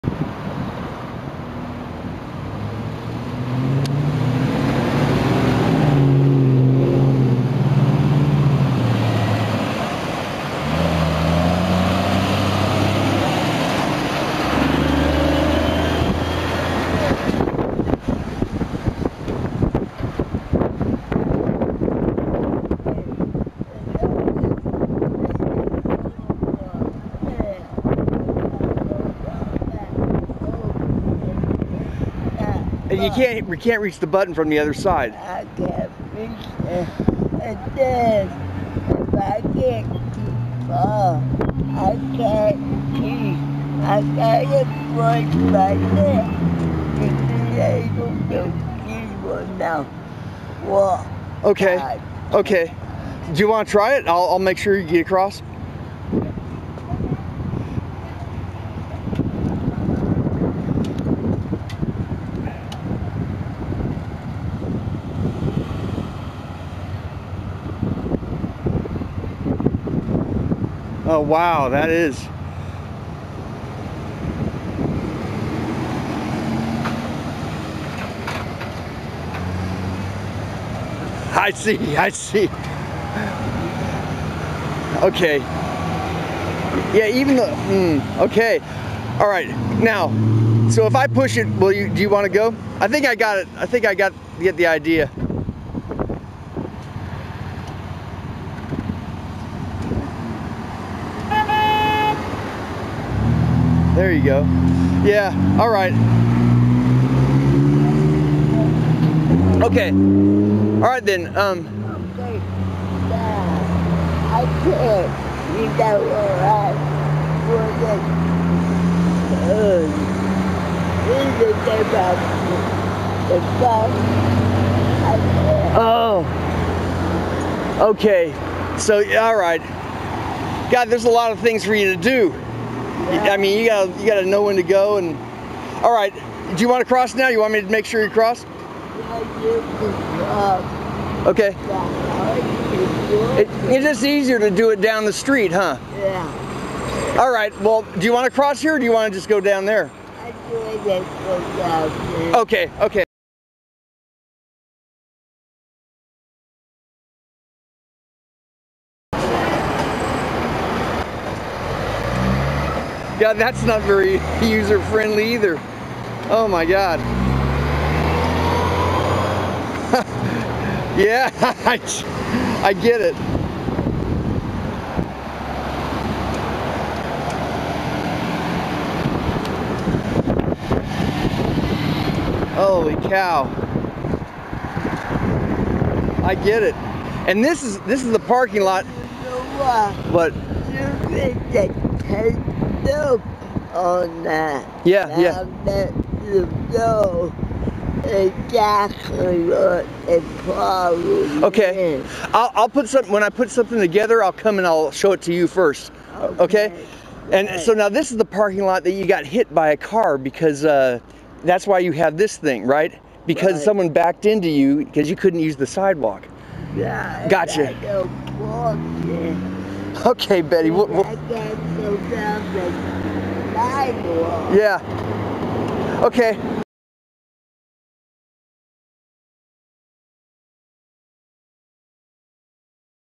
Project Betty
How safe are our streets?  And what about crosswalks... Does one feel safe using a certain crosswalk to cross a heavily traveled road?  Is the design easy to use and understand for everyone?  I had seen Betty scooting around town before but when I saw her traveling a quarter of a mile, through a different crosswalk, and backtrack to the place she has lunch, I had a suspicion that she didn't like to use the closer crosswalk.  I presented this video to the County and some changes have been made to that corner.